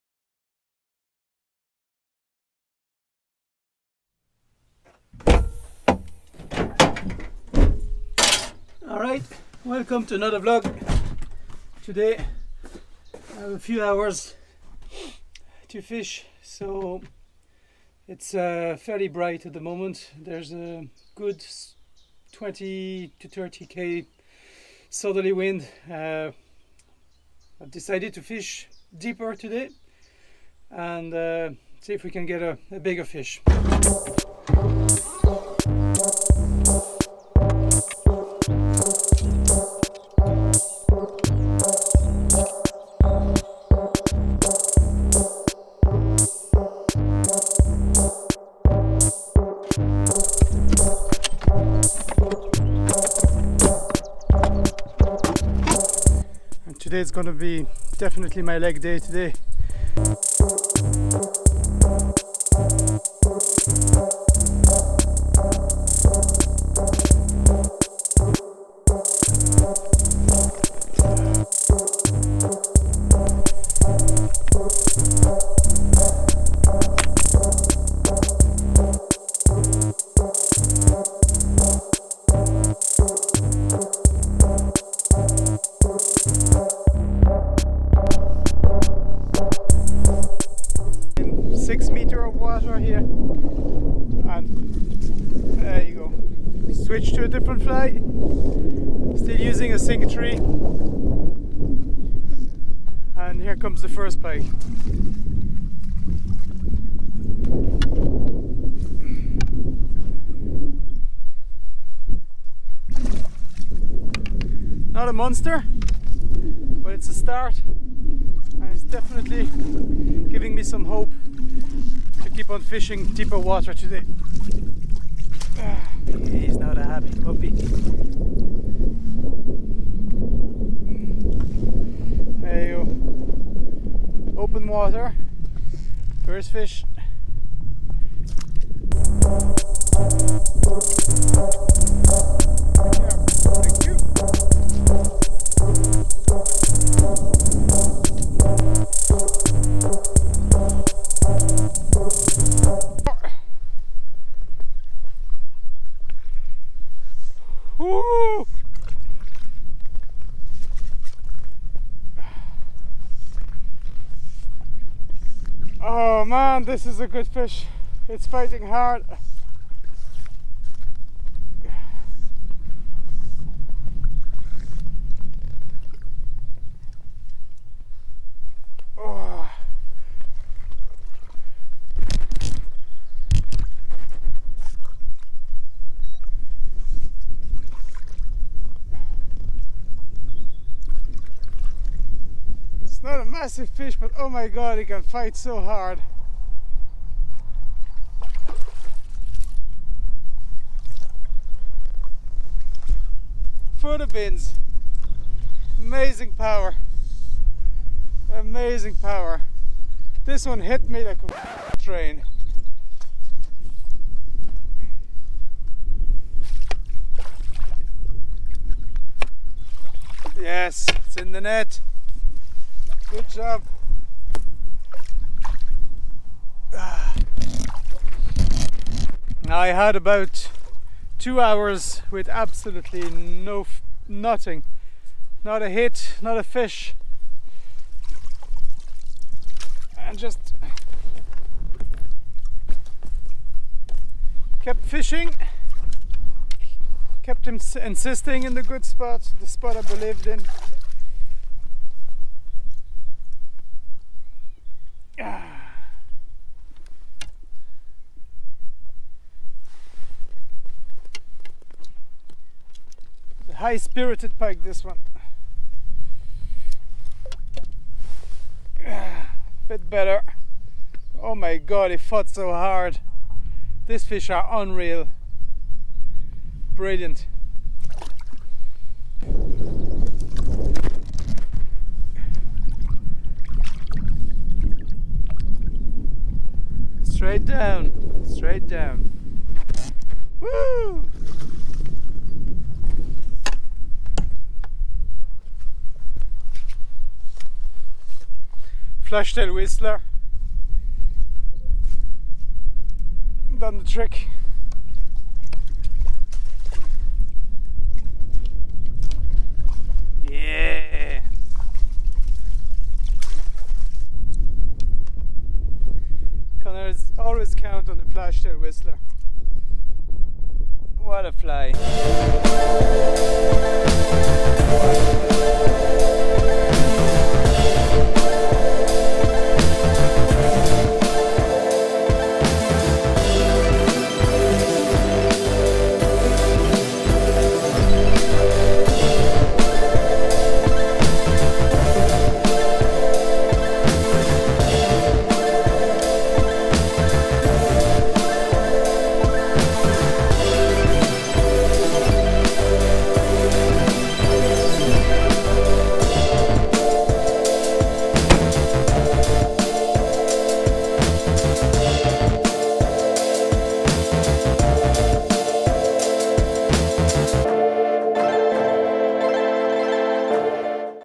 Vlog#11 : Flyfishing for pike in deeper water.
A quick morning session, with bright conditions and high pressure i decided to fish a deeper lake today to find fish that wanted to feed.